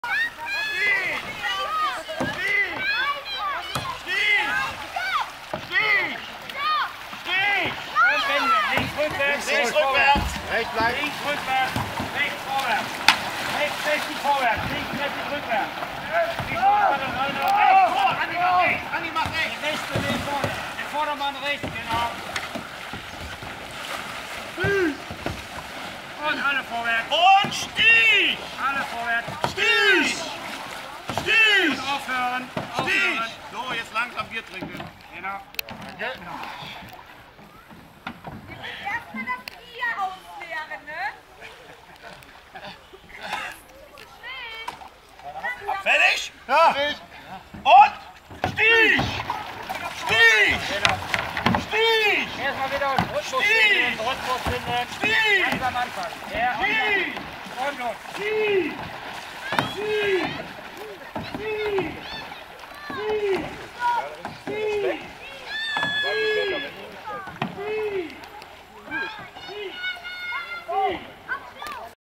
Allzeit bereit beim Canadierrennen Wevelinghoven 2016
Das Boot von "Allzeit bereit" beim Canadierrennen in Wevelinghoven am 11. Juni 2016.